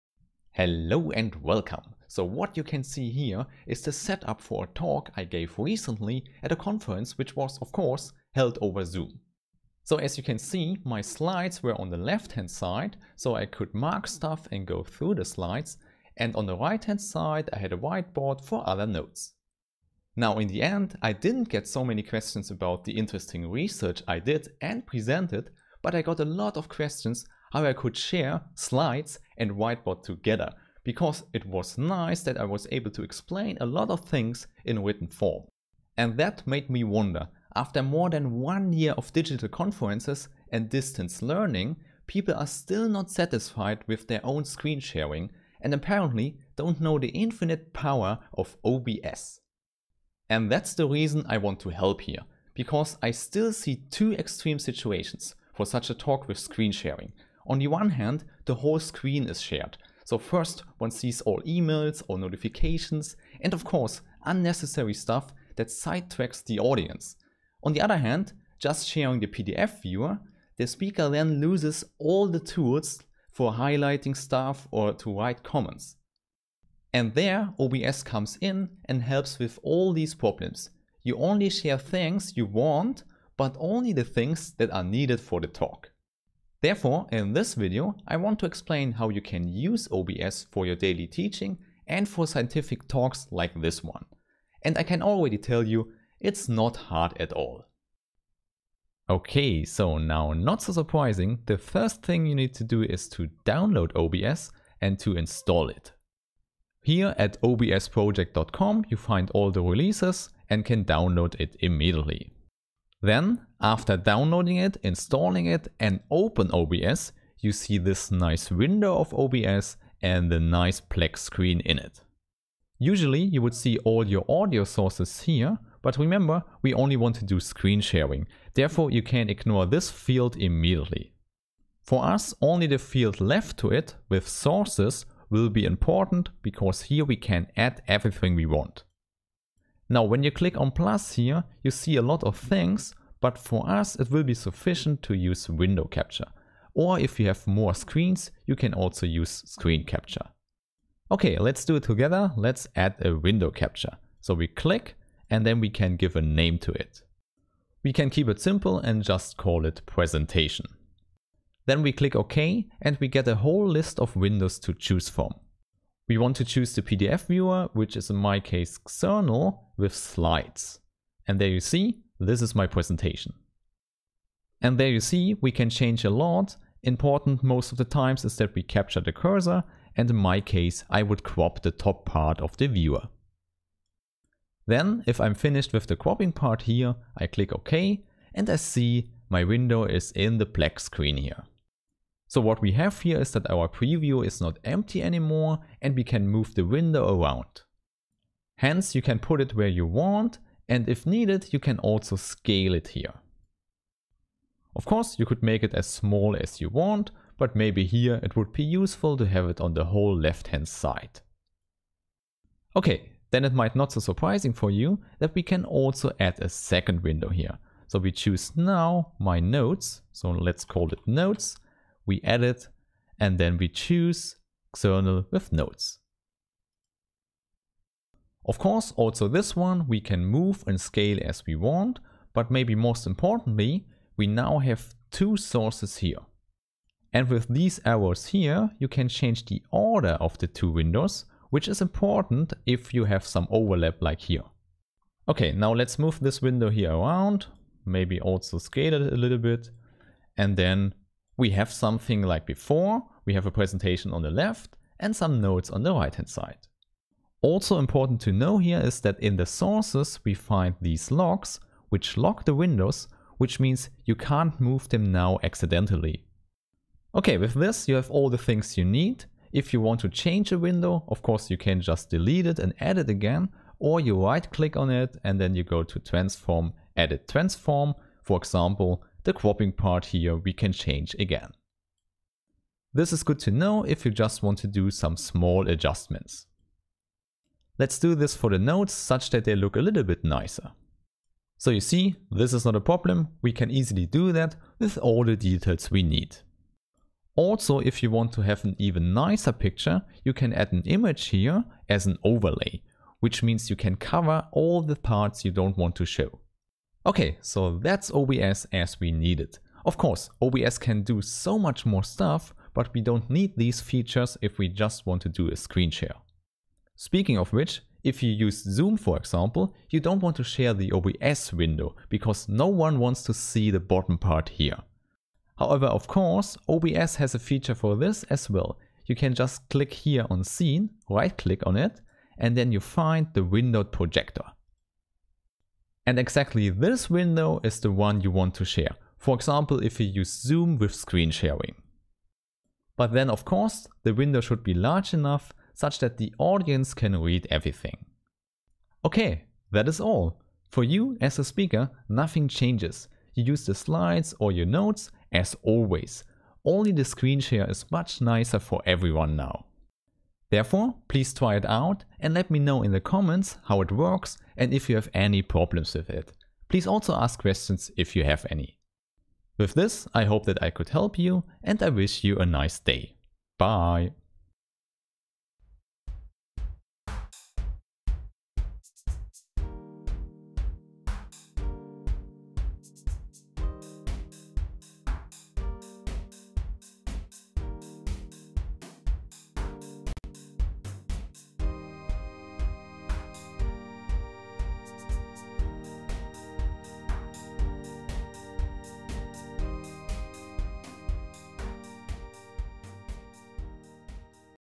Explaining Mathematics with OBS
OBS is a powerful program. Combining it with online conferences has large advantages for everyone. So please use this nice software :)